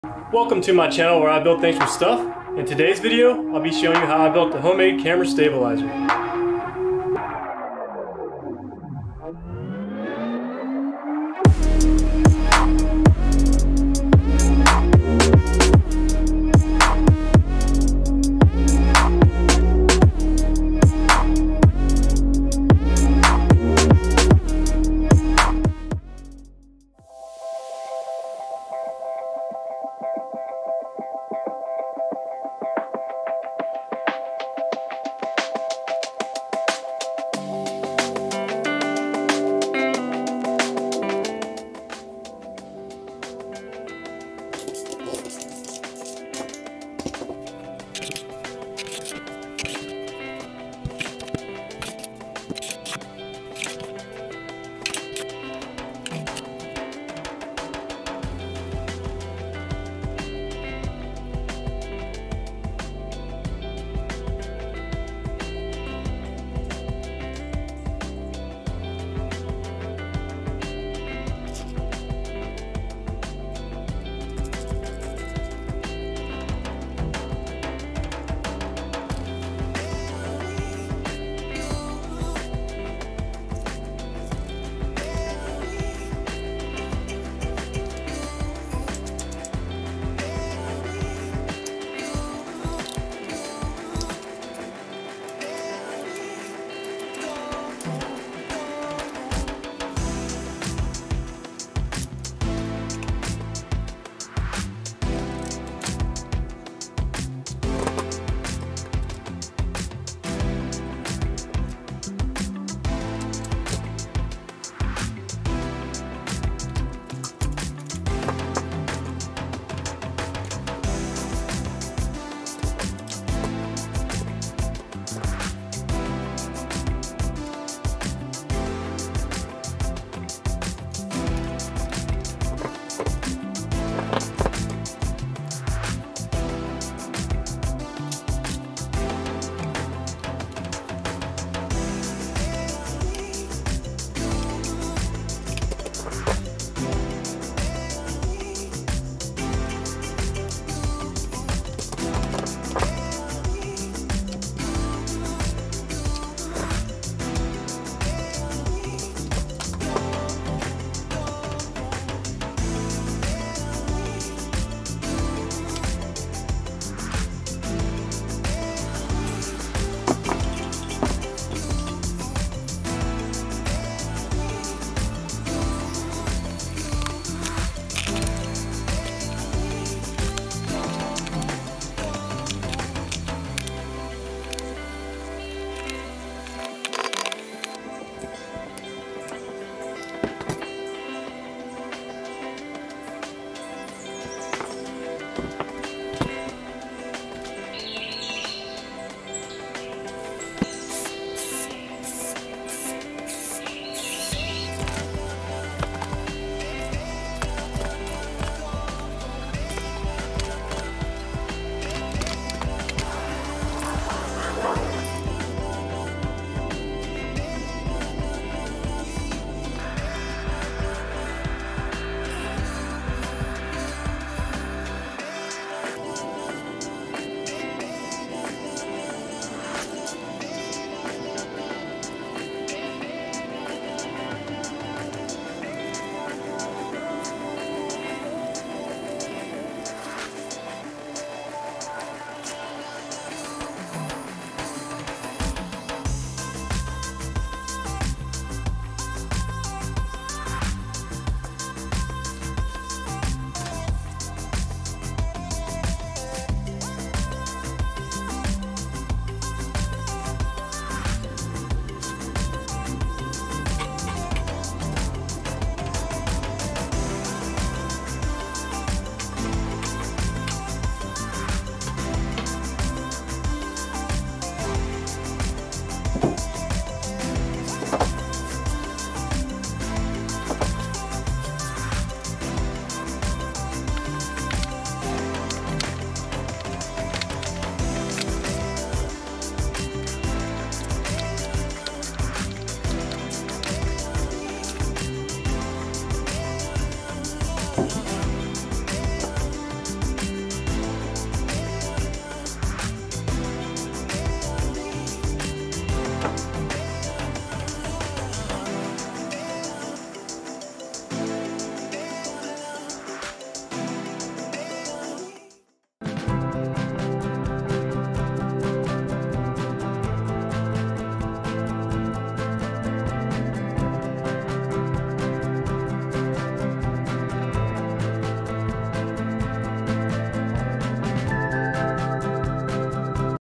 DIY Camera Stabilizer Handle Grip - How To Make
This video shows how I built a homemade camera stabilizer rig from PVC pipe! This is an inexpensive solution to hold / maneuver a video camera and hopefully create more stable videos as opposed to holding the camera by hand. This is by no means a Gimbal stabilizer with a gyroscope, just a simple way to control your camera. Works for my DSLR camera but should fit all types of cameras with a 1/4" connection.
Material list and tools used are included in the video.